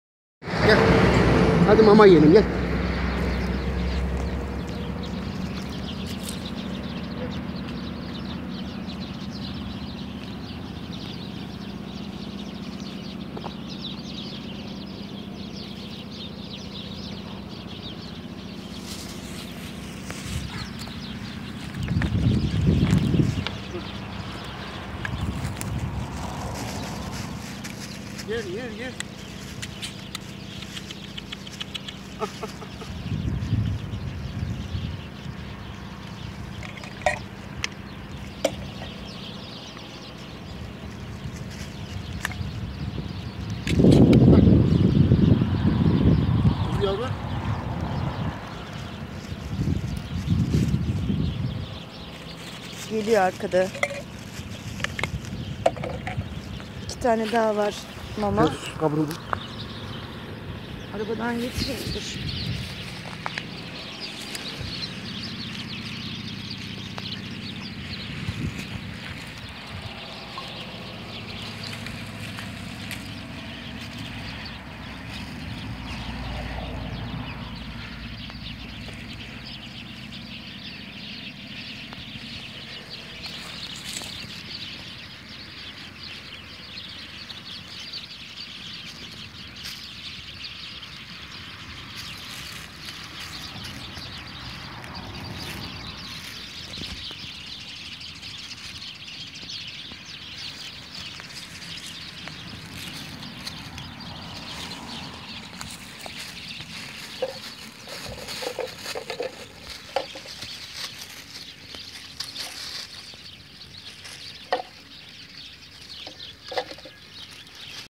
8 Ekim 2016
Bugün Bebekler ve anneyi Güler Inal hanımın arac ve konserve süt desteği ile doyurduk